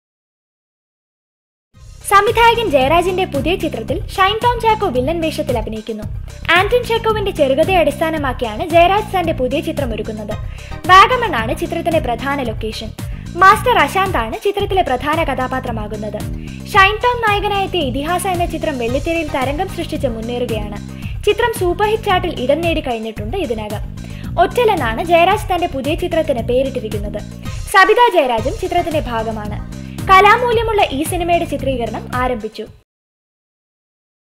Shine Tom Chacko as villain in Jayaraj's Ottal
Ace director Jayaraj's next movie, "Ottal" which is loosely based on Anton Chekkov's short story Vanka. New face Ashanth will play the child protagonist and Shine Tom Chacko plays the villain in the film.  Sabitha Jayaraj is also a part of this sync sound film, the shoot of which is currently progressing at Vagamon,.  Shine Tom's last film 'Ithihasa' saw him play the hero for the first time which is running successfully at theatres. Filmmaker Jayaraj is known for his art house films and has bagged National and State Awards for his works.